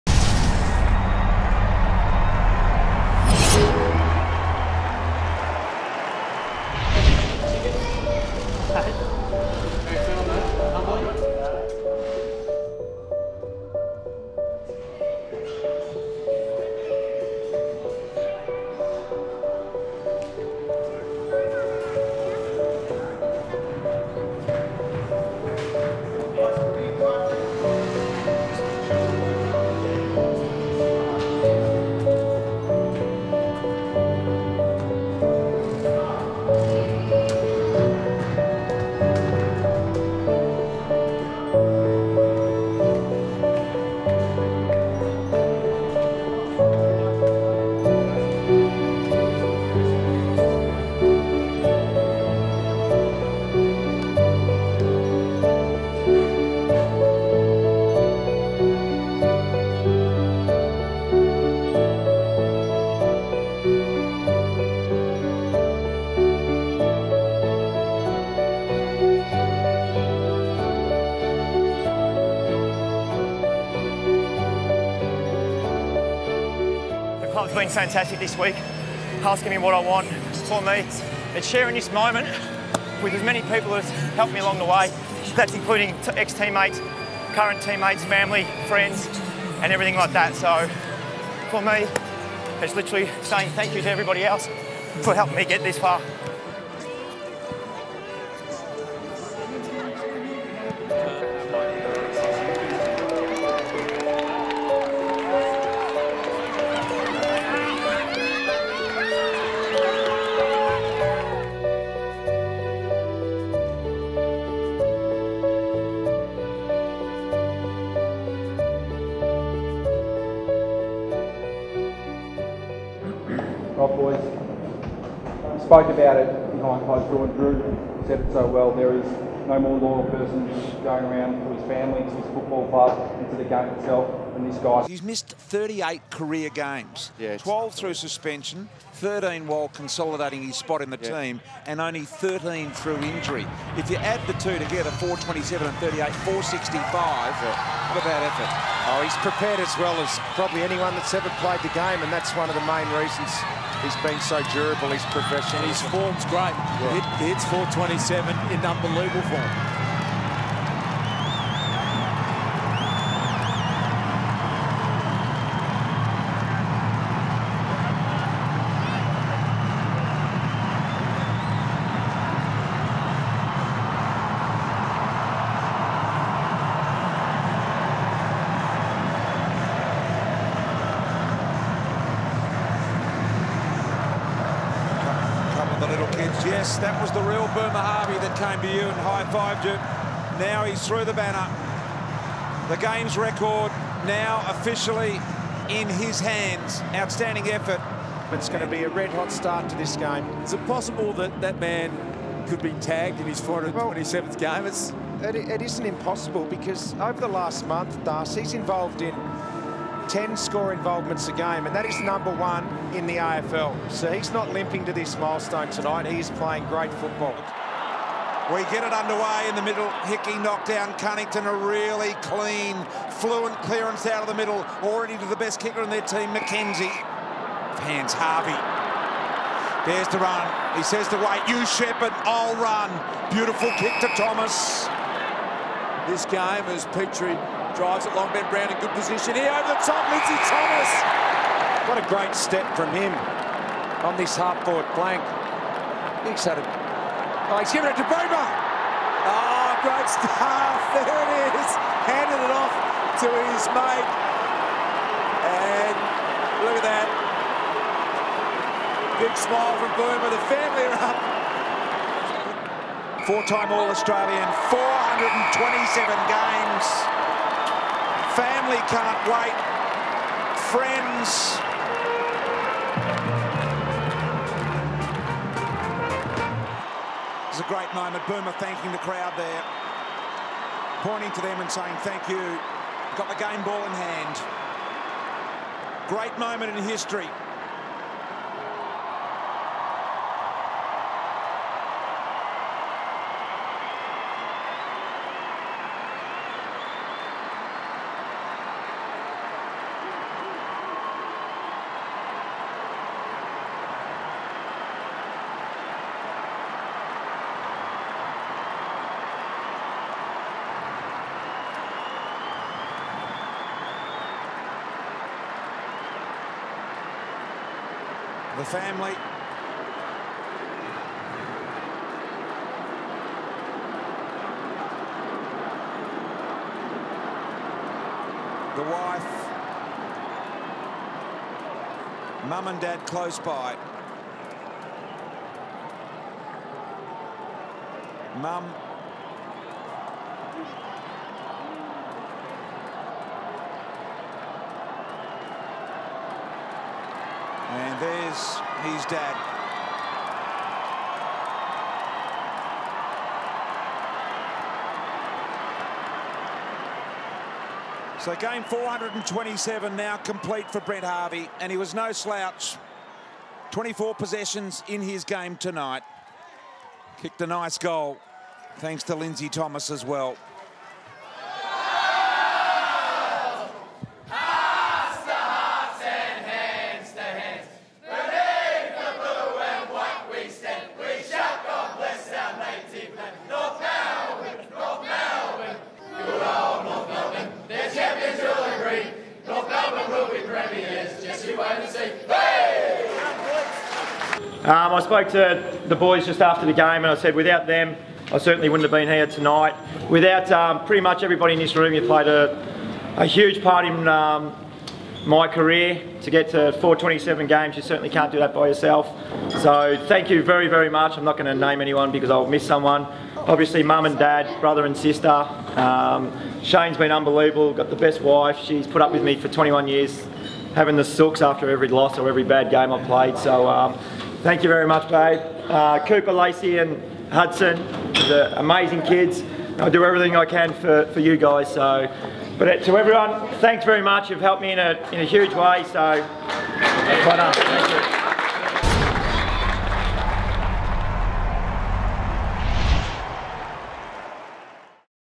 Boomer427 - Brent Harvey's big day
Follow a magical milestone as Brent Harvey breaks the AFL/VFL games record.